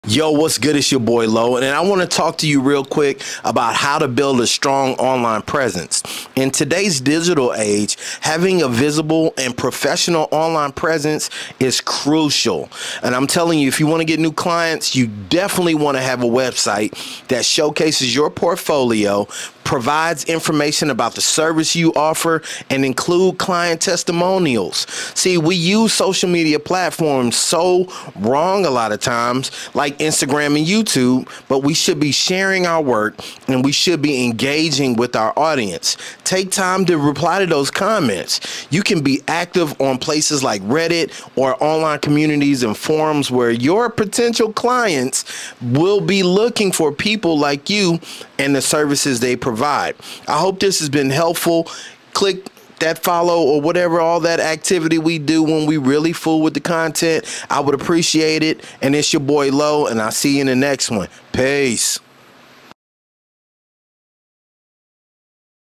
Build a Strong Online Presence
Build a strong online presence: In today's digital age, having a visible and professional online presence is crucial for attracting potential clients. Create a website that showcases your portfolio, provides information about the services you offer, and includes client testimonials. Use social media platforms like Instagram and YouTube to share your work and engage with your audience. Be active on relevant online communities and forums where potential clients might be seeking videographers.